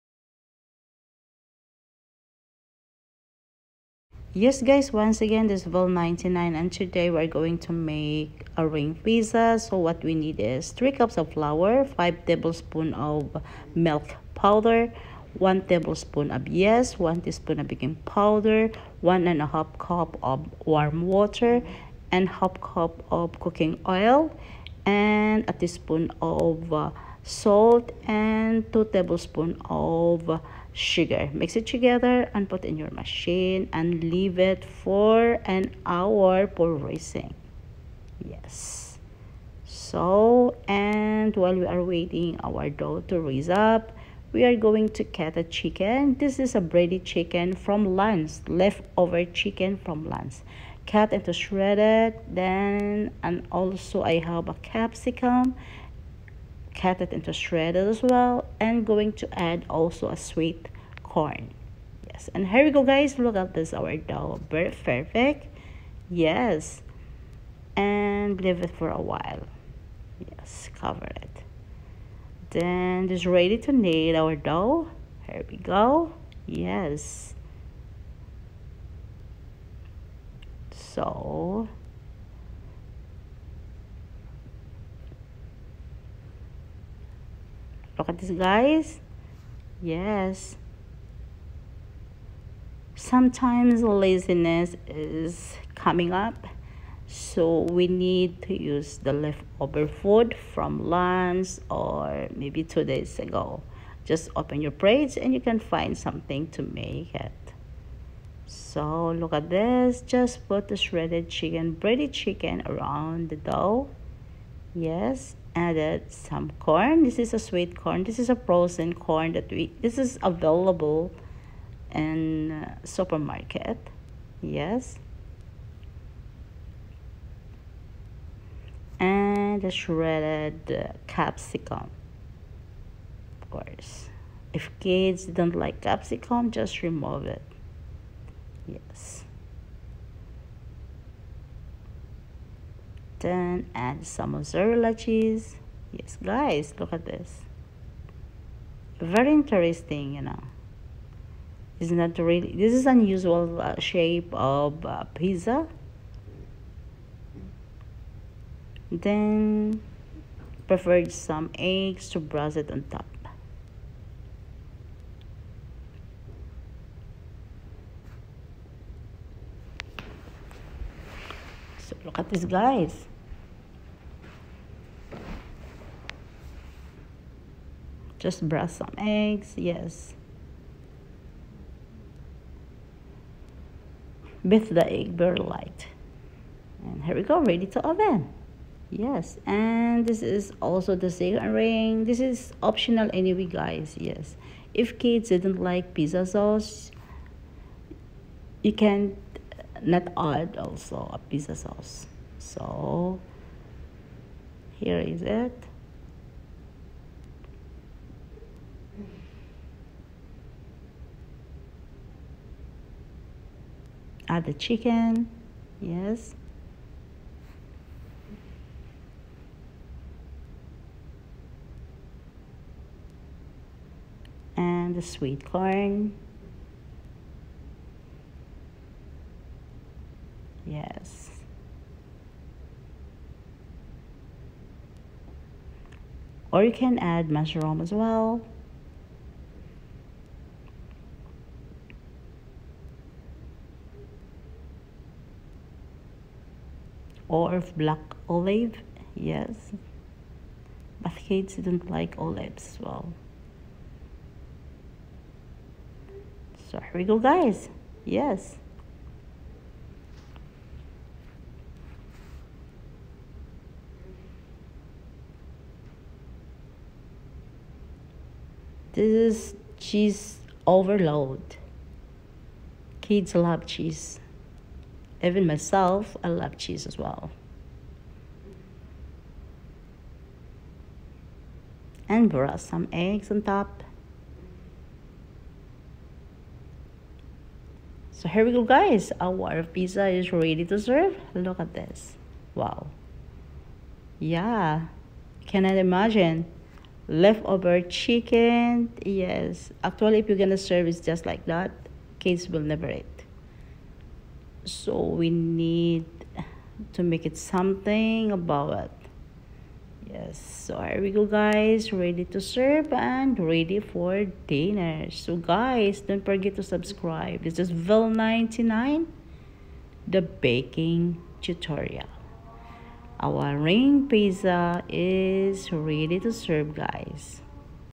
Left over chicken turn it into a delicious Ring pizza watch this..
This easy pizza ring is perfect dinner for  busy night it take family nights to a whole new level your kids will lobe making this with you it takes a minutes to trow together and you can add all your favorite ingredients and the left over chicken from lunch ..